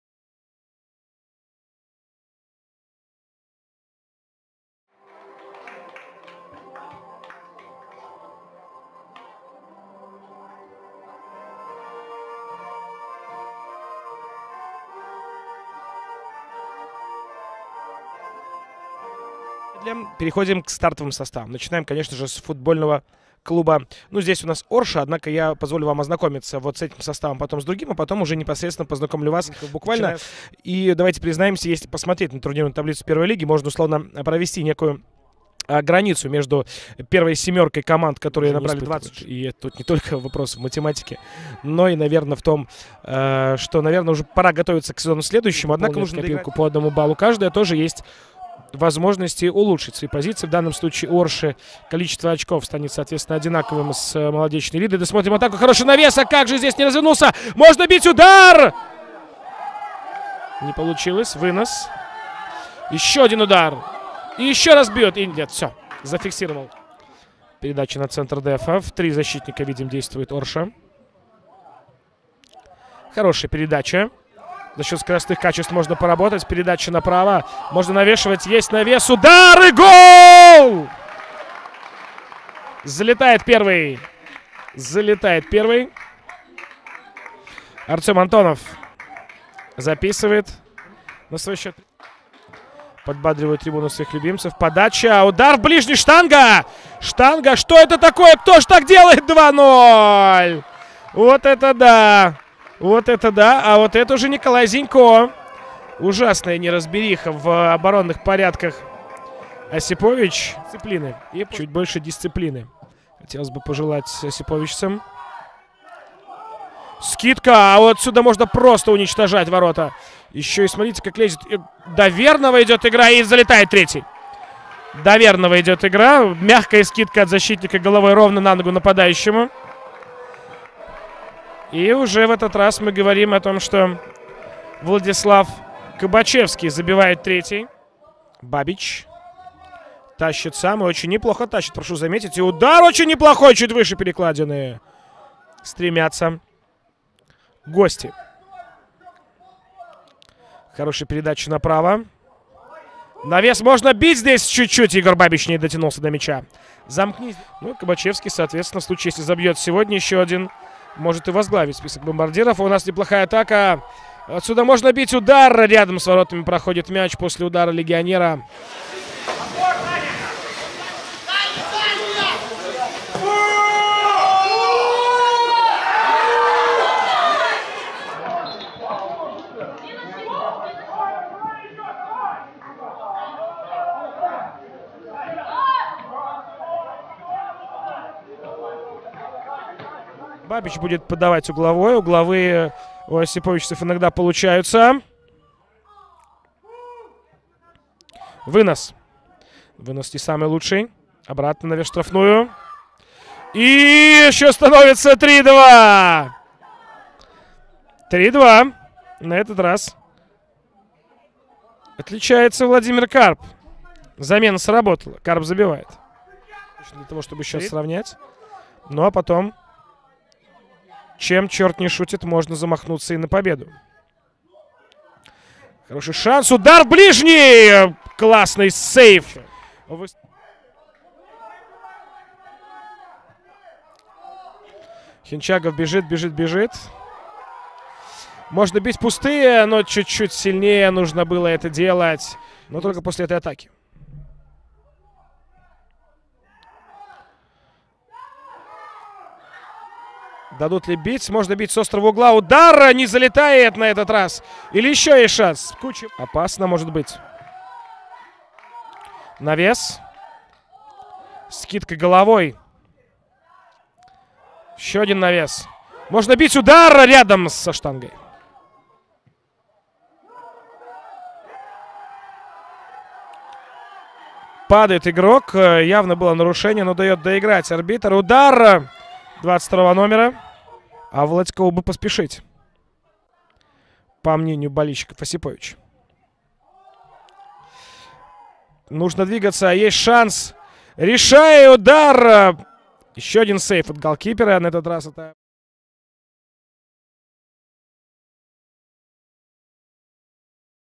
Обзор матча Орша — Осиповичи
Первая лига, 19 тур. Орша — Осиповичи — 3:2
Голы: 1:0– Антонов (9), 2:0 – Зенько (13), 3:0– Кабачевский (23),
3:1– Потершук (53), 3:2– Карп (58).